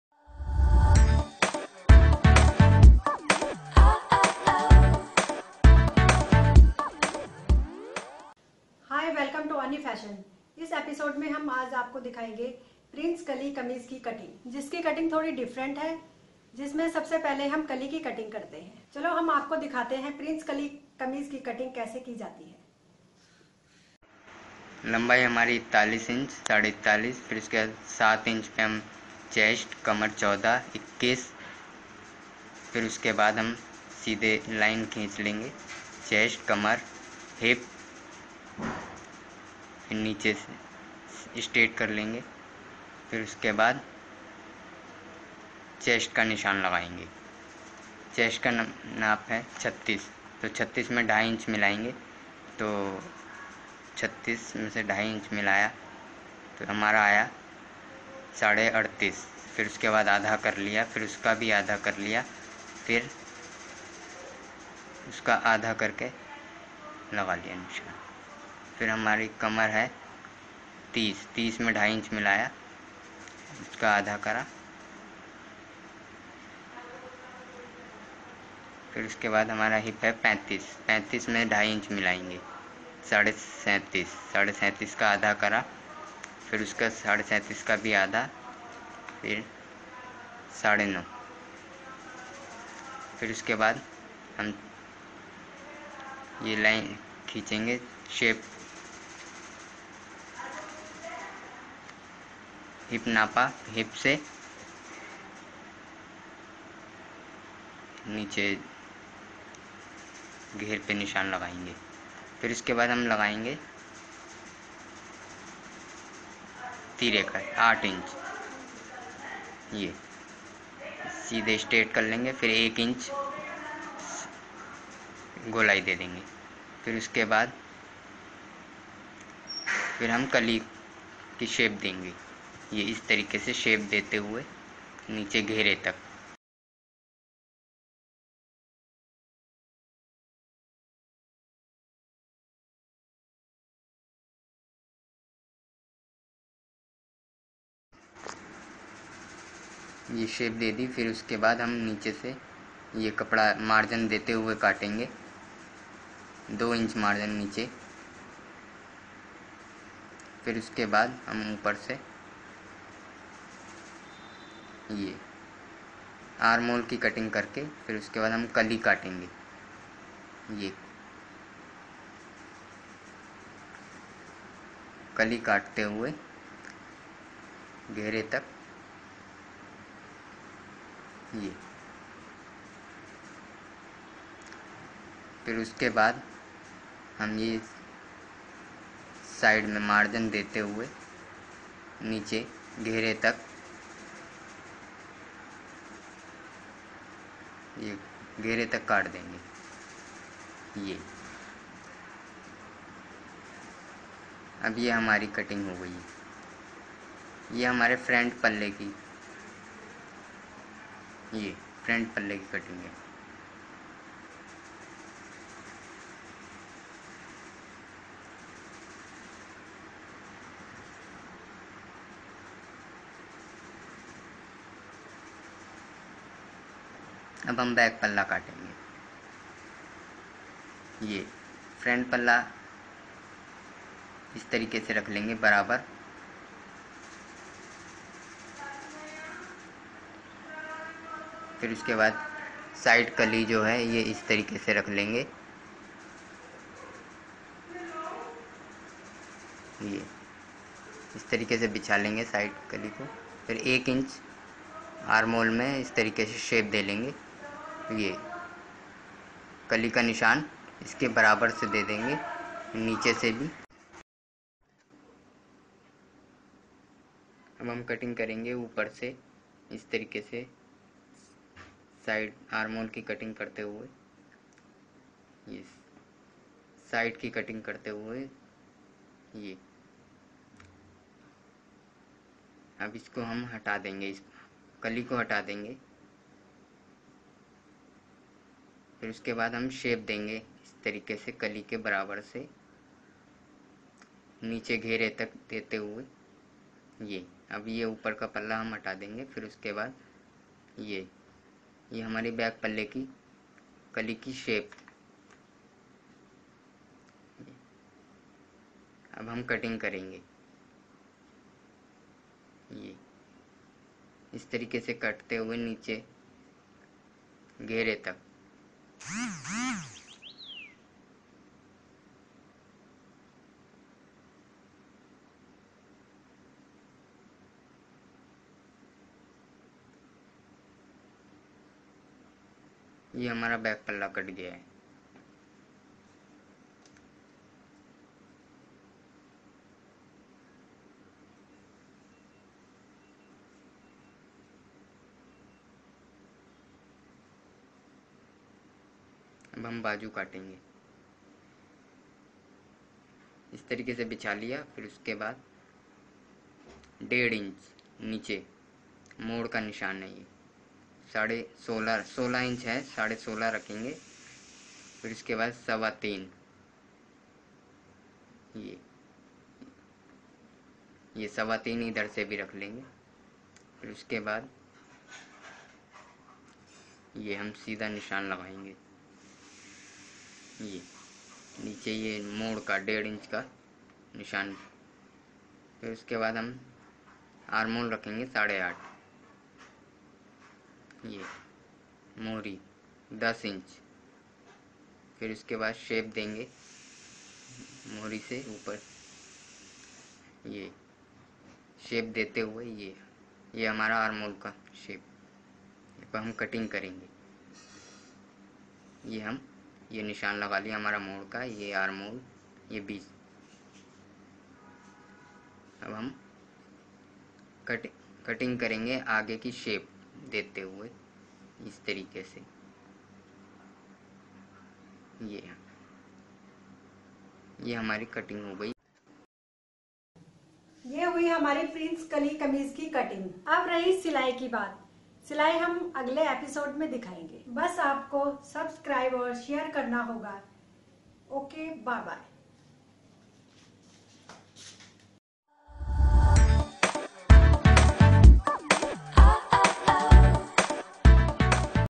Prince Kaali kamije cutting part 1
लेडिस प्रिंस कली कमीज कटिग सरल व आसान तरीका,भाग,1,